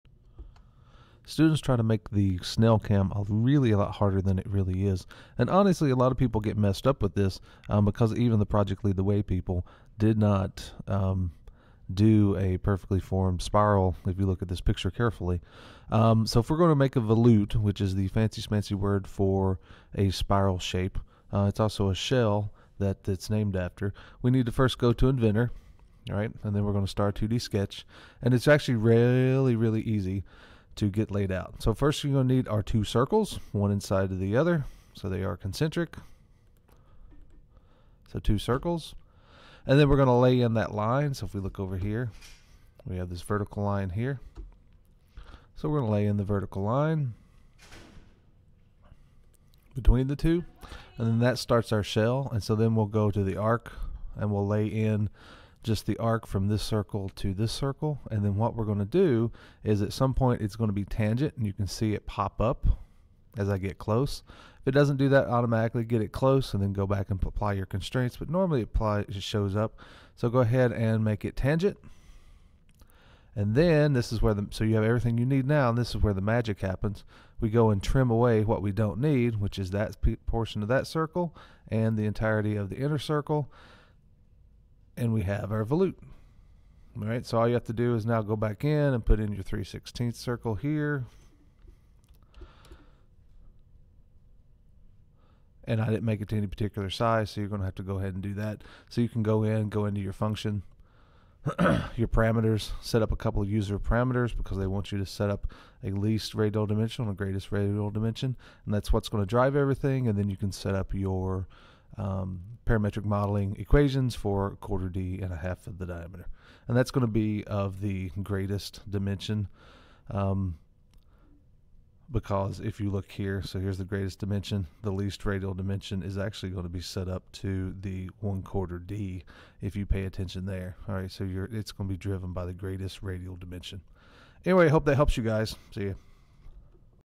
Activity 8.1 Forming the snail shaped cam.
How to build the snail shaped cam for the parametric modeling assignment.